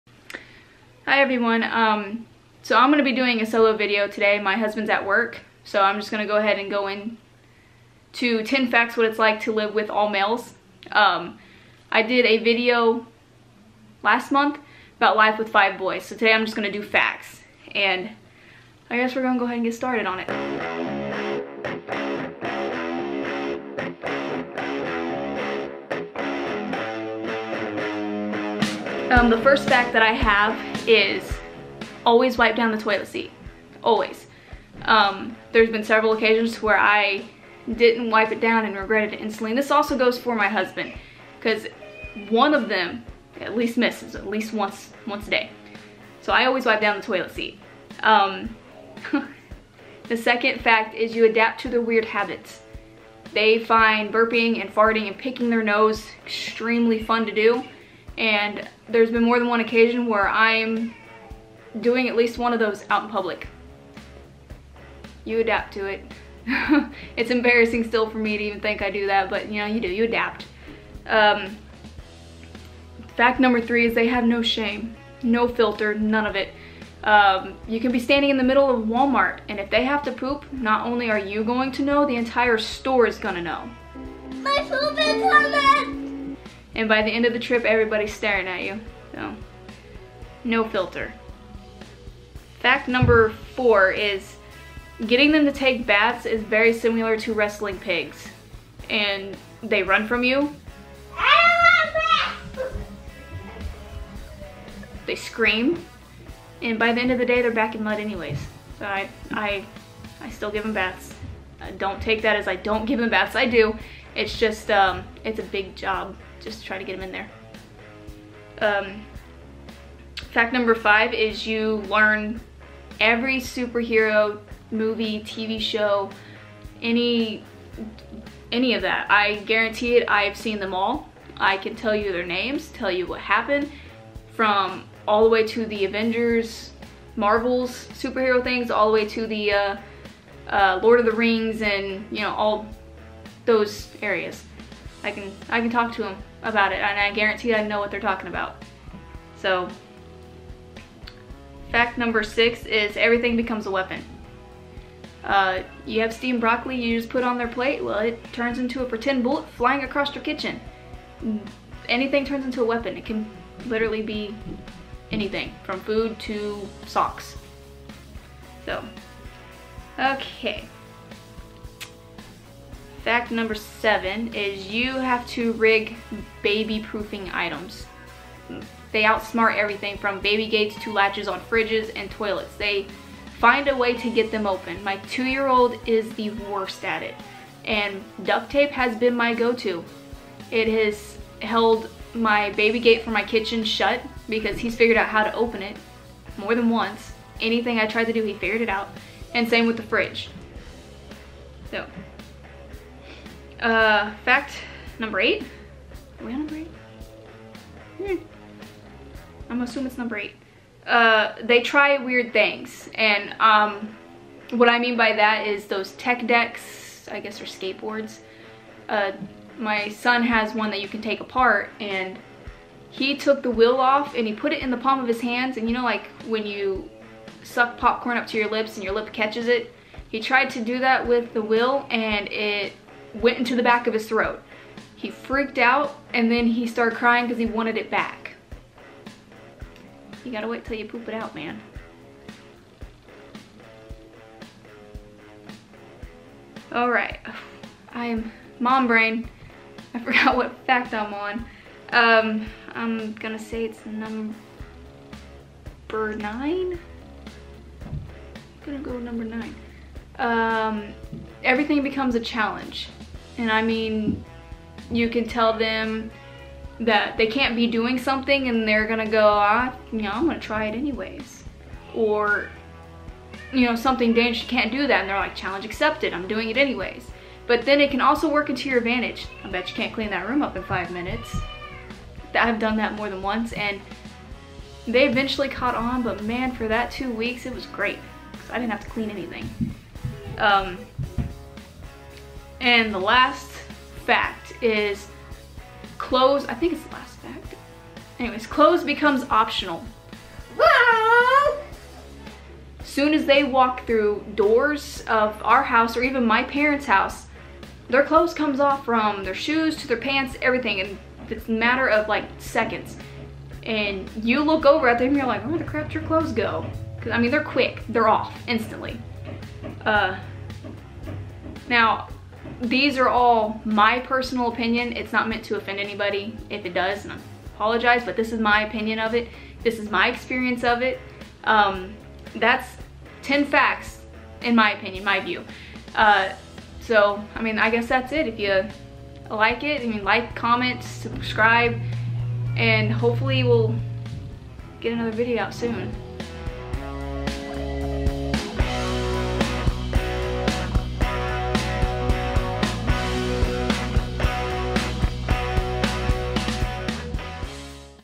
10 facts about boys | mom of only boys | VLOG #21
10 facts you should know about raising boys. If you are a parent of a boy, expecting a boy, or are just curious about raising these monster you have come to the right place. I share my experience raising 5 boys, so needless to say I have some experience in it.